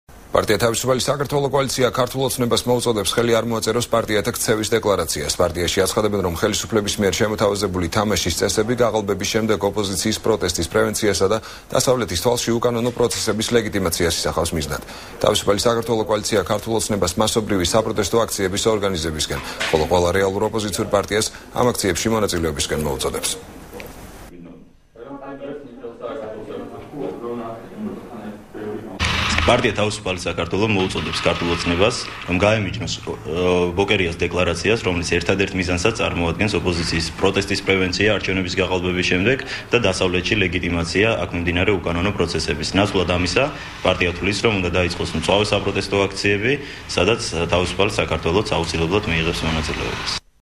ახალი 6 | თავის. საქართველოს მოწოდ. | 21.08
პარტია „თავისუფალი საქართველო" კოალიცია „ქართულ ოცნებას" მოუწოდებს ხელი არ მოაწეროს პარტიათა ქცევის დეკლარაციას. პარტიაშ აცხადებენ, რომ ხელისუფლების მიერ შემოთავაზებული თამაშის წესები, გაყალბების შემდეგ ოპოზიციის პროტესტის პრევენციასა და დასავლეთის თვალში უკანონო პროცესების ლეგიტიმაციას ისახავს მიზნად. თავისუფალი საქართველო კოალიცია ქართულ ოცნებას მასობრივი საპროტესტო აქციების ორგანიზებისკენ, ხოლო ყველა რეალურ ოპოზიციურ პარტიას ამ აქციებში მონაწილეობისკენ მოუწოდებს.